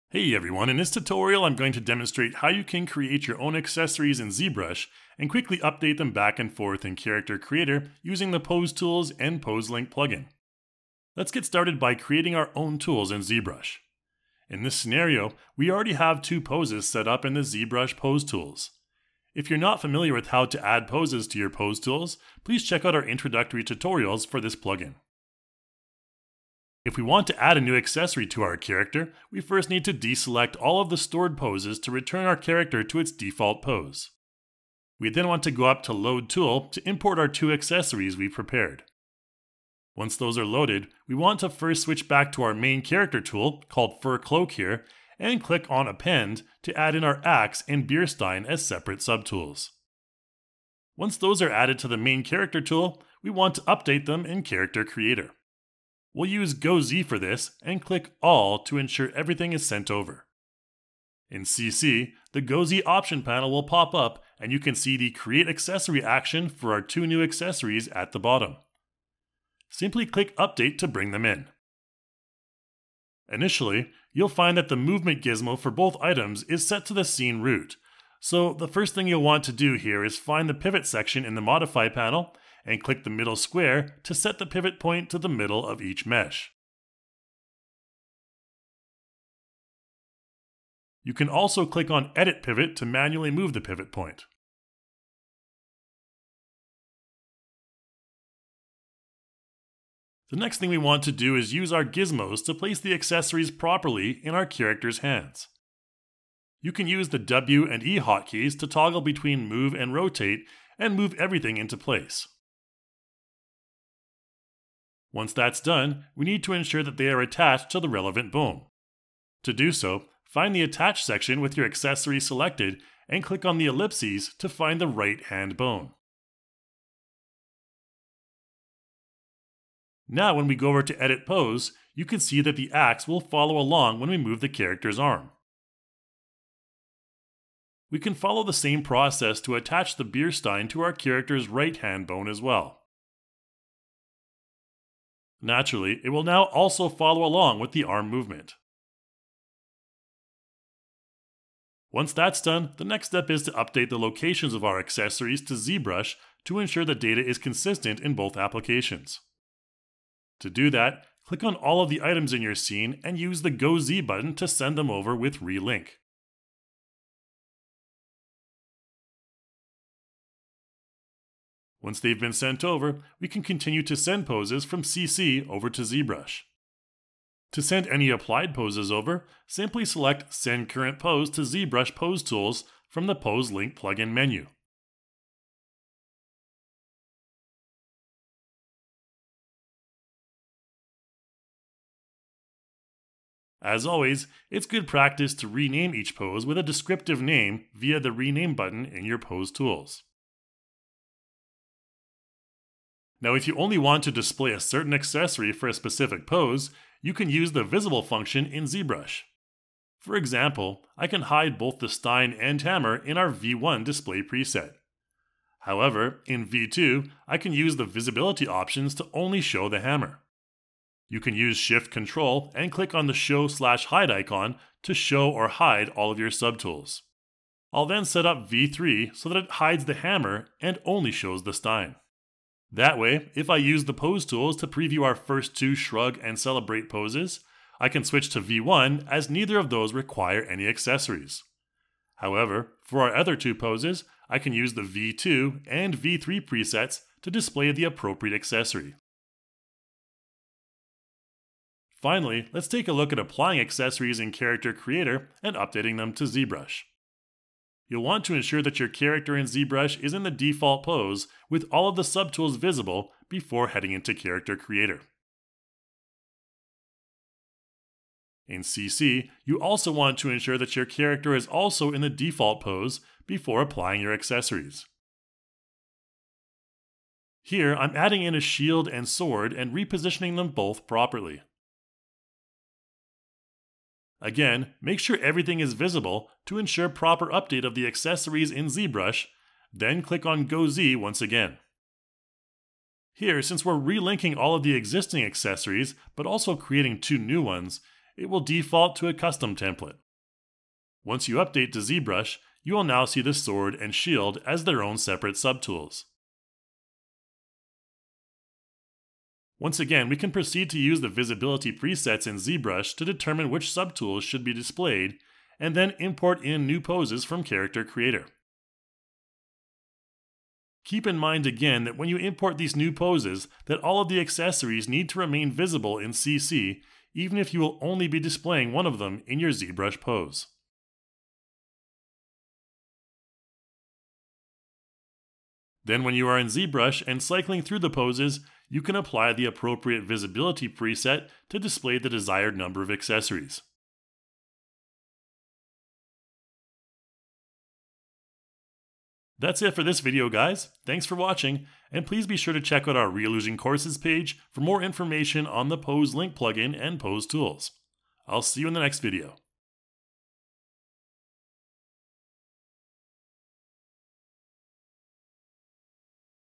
Creating Accessories for Postured Characters | Pose Tools & Pose Link Tutorial
The Pose Link plug-in and Pose Tools provide an optimized pipeline between Character Creator and ZBrush for artists who want a more efficient workflow.  In this tutorial, you’ll learn about how you can add accessories to your character and get them all set up in CC, and use the different poses and visibility presets to display the desired pose/accessory combination.

00:00 Intro
00:10 Creating Accessories in ZBrush
04:15 Applying Accessories in Character Creator

▪ Character Creator to ZBrush Posing pipeline (Pose Link & Pose Tools plugin)

✦ONLINE HELP
[Character Creator]